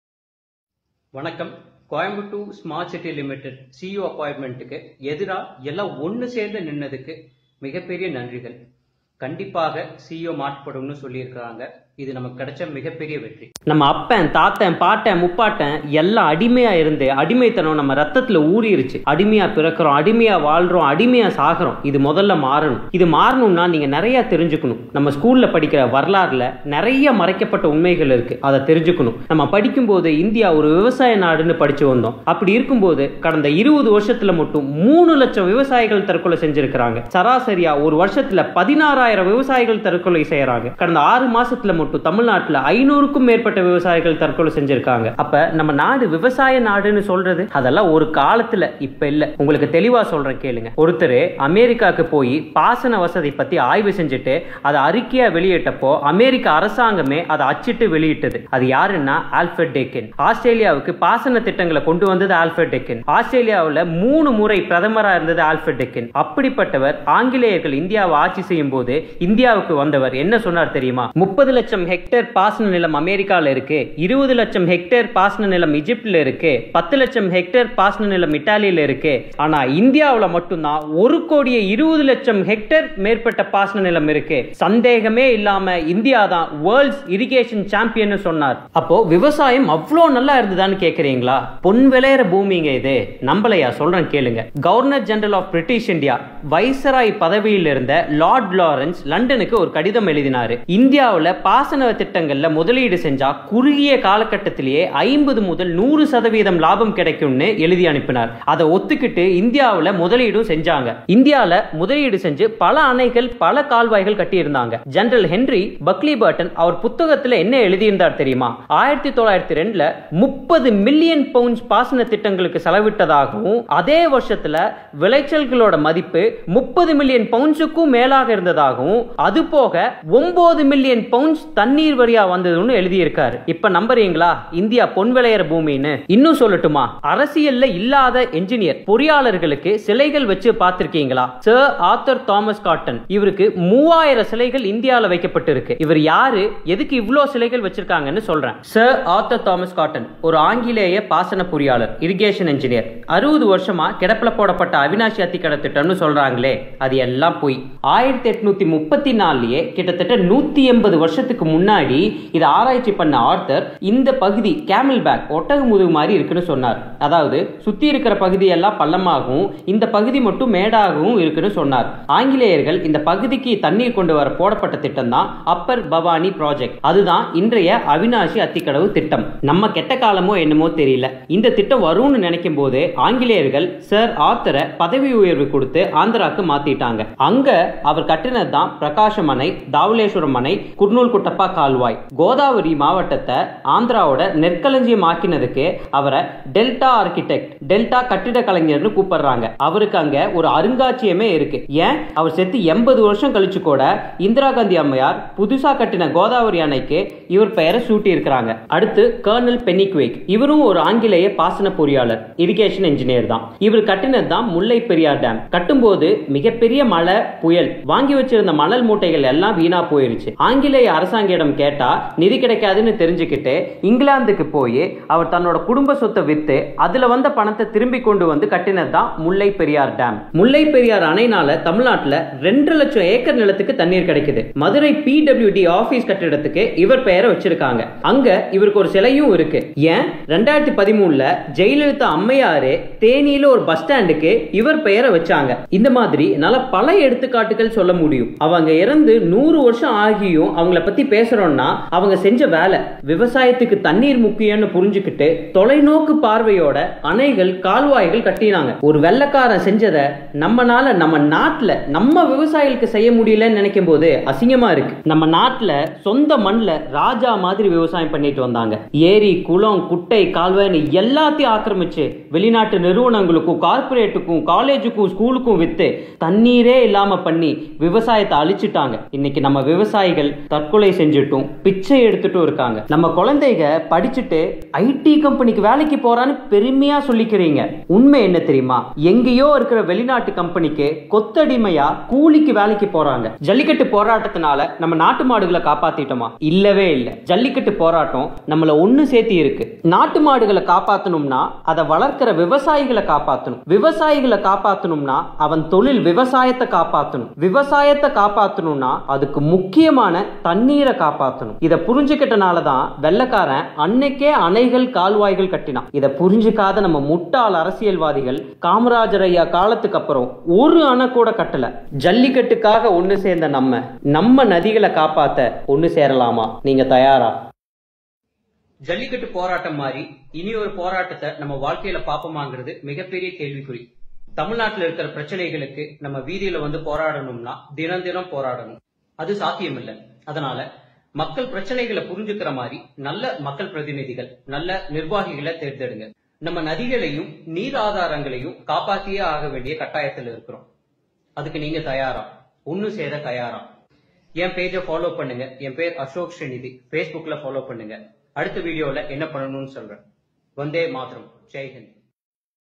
Water Resources
எத்தனை காலம் அடிமையாக இருக்க போறீங்க. ?
60 வருடமாக ஒரு அணை கூட கட்டவில்லை !!